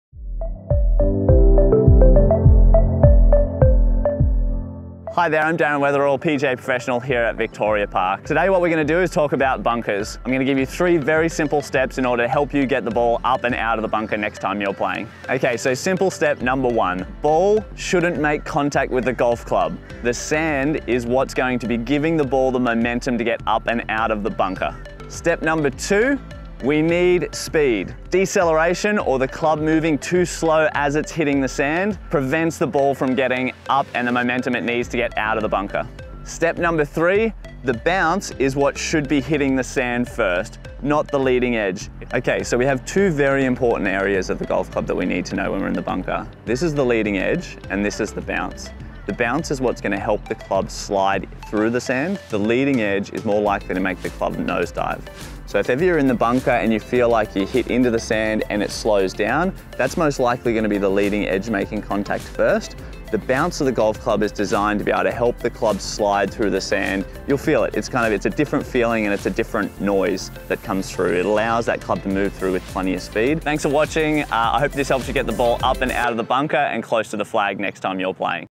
How to get out of Bunkers
Victoria Park Golf Complex engaged us to produce a series of 10 golf coaching videos to drive business to their centre. Featured here is Darren Weatherall a PGA professional running through some tips on how to improve your chances of getting out of bunkers